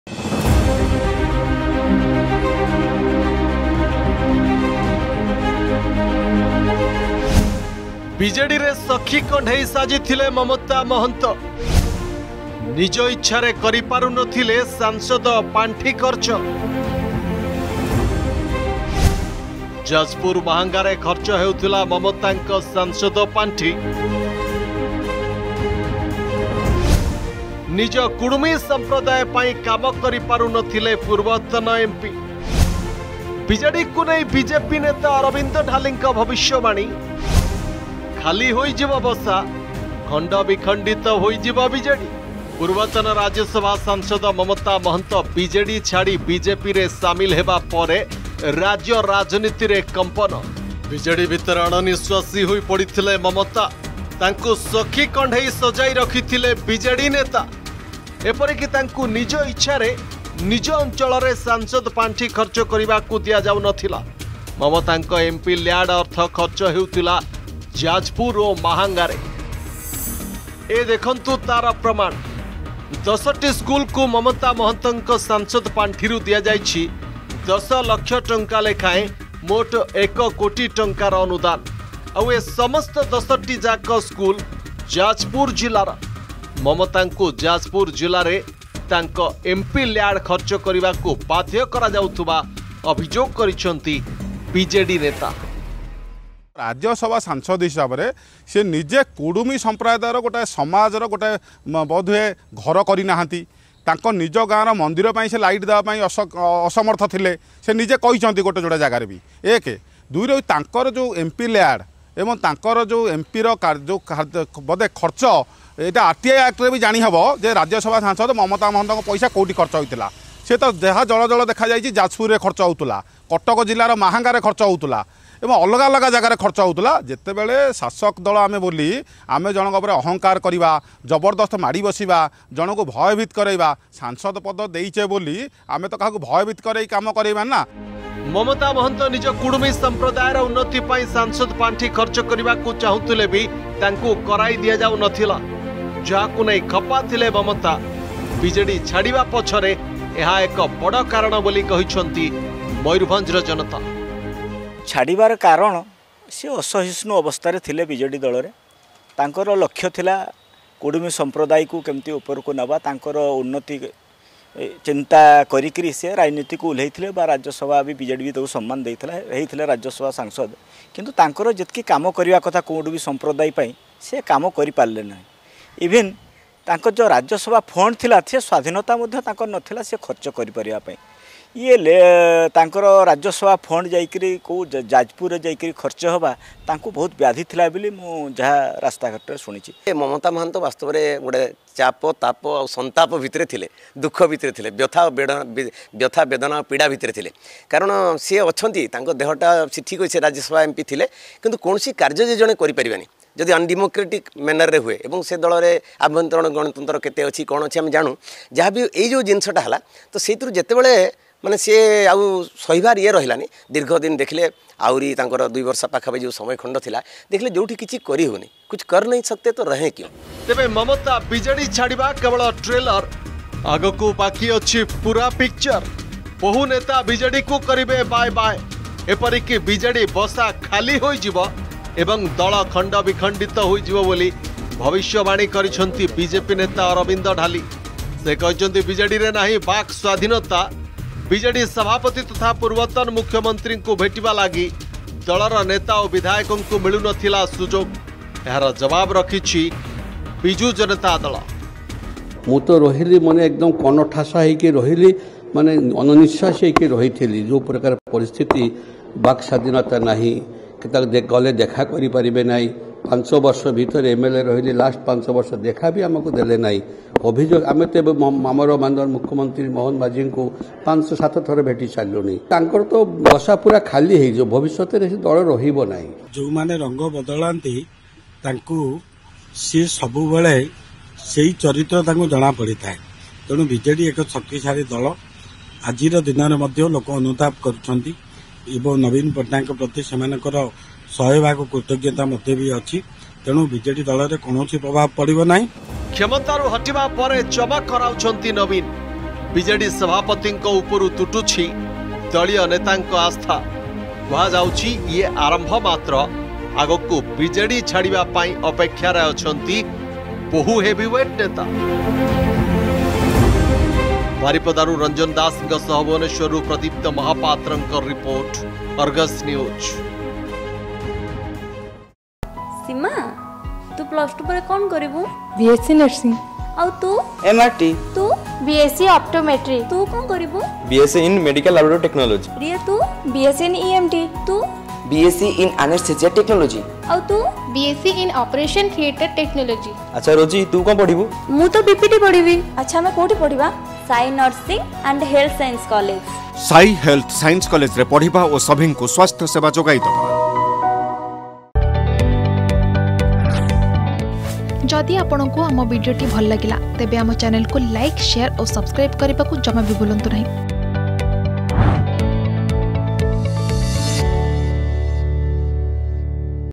ବିଜେଡିରେ ସକ୍ଷୀ କଣ୍ଢେଇ ସାଜିଥିଲେ ମମତା ମହନ୍ତ || Mamata Mahanta
ବିଜେଡିରେ ସକ୍ଷୀ କଣ୍ଢେଇ ସାଜିଥିଲେ ମମତା ମହନ୍ତ । ନିଜ ଇଚ୍ଛାରେ କରିପାରୁନଥିଲେ ସାଂସଦ ପାଣ୍ଠି ଖର୍ଚ୍ଚ । ଯାଜପୁର, ମାହାଙ୍ଗାରେ ଖର୍ଚ୍ଚ ହେଉଥିଲା ମମତାଙ୍କ ସାଂସଦ ପାଣ୍ଠି । ନିଜ କୁଡୁମି ସମ୍ପ୍ରଦାୟ ପାଇଁ କାମ କରିପାରୁନଥିଲେ ପୂର୍ବତନ ଏମପି । ବିଜେଡିକୁ ନେଇ ବିଜେପି ନେତା ଅରବିନ୍ଦ ଢାଲିଙ୍କ ଭବିଷ୍ୟବାଣୀ ।